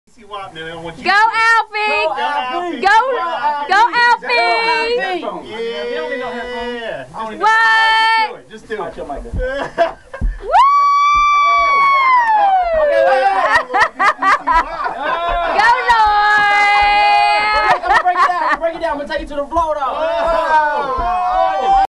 Lloyd LIVE in studio with OC104 Part 2
R&B superstar Lloyd stopped by OC104 in Georgetown, De.